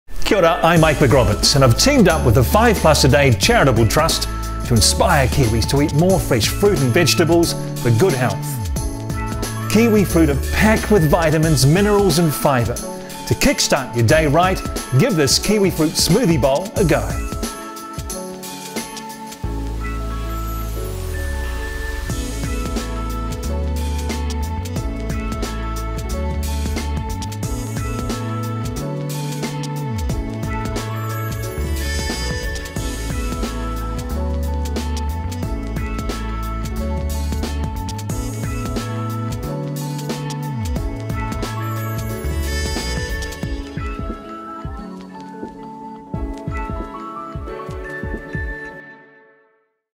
Best Green Smoothie Bowl
With kiwifruit, spinach, bananas and more.

This video shot for the 5+ A Day Charitable Trust to inspire New Zealanders to eat more fruit and vegetables. This series was fronted by Mike McRoberts, a well-known NZ journalist and TV presenter.